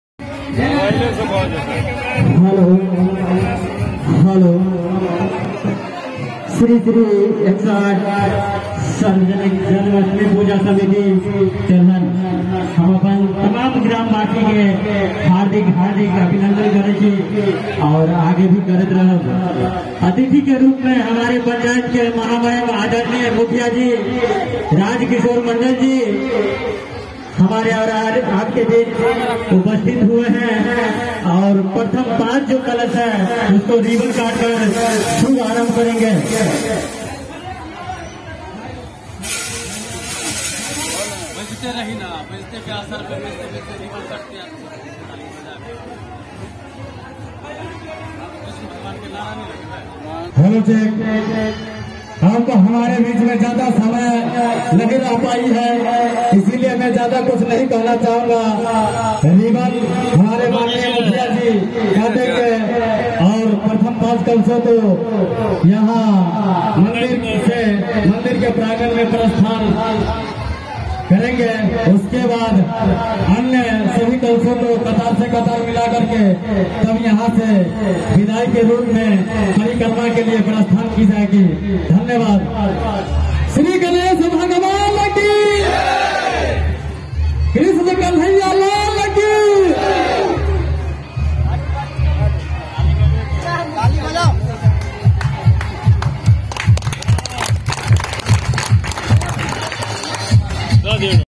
mere gaon ke Janmashtami mein aapka swagat hai Kalash Shobha Yatra ka shubharambh#tethan vlogs
mere gaon ke Janmashtami mein aapka swagat hai Kalash Shobha Yatra ka shubharam Shri mananiy mahamahim hamare Panchayat ke mukhiya ji dwara rivana kat ke shubharambh Kiya jata hai is samay pure gaon ke shradhalu bhaktgan upsthit aur hamare commity ke sabhi sadasya upsthit rahte hain#tethan vlogs